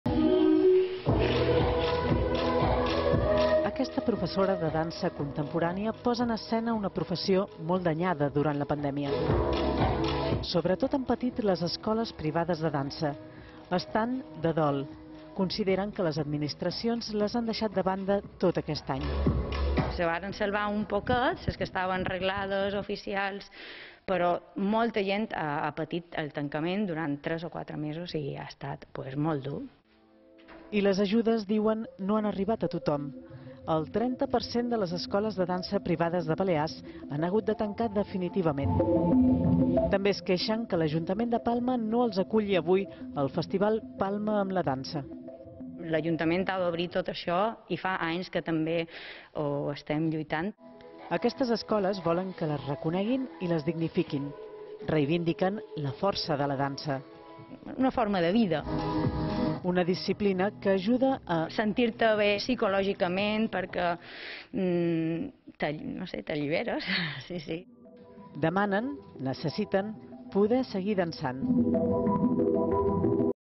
La crisi acaba amb el 30% de les escoles privades de dansa
L'Associació que les agrupa no se sent "amb forces" de celebrar el Dia Internacional de la Dansa